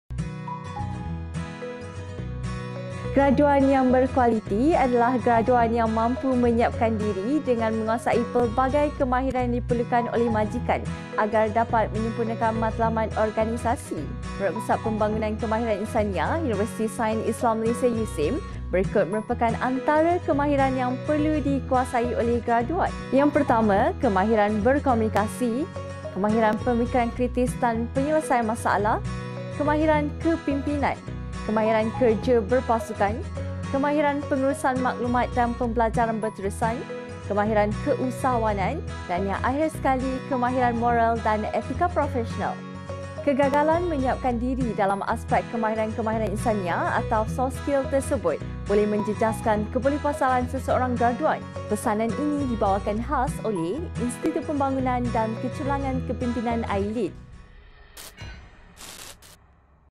Info Khas : Kemahiran Insaniah (Softskill) Bagi Menempuh Alam Pekerjaan
Apa kemahiran yang graduan perlukan?

1. Kemahiran Berkomunikasi.
2. Kemahiran Pemikiran Kritis.
3. Kemahiran Penyelesaian Masalah.
4. Kemahiran Kepimpinan.
5. Kemahiran Kerja Berpasukan.
6. Kemahiran Pembelajaran Berterusan.
7. Kemahiran Pengurusan Maklumat.
8. Kemahiran Keusahawan, Moral dan Etika Profesional.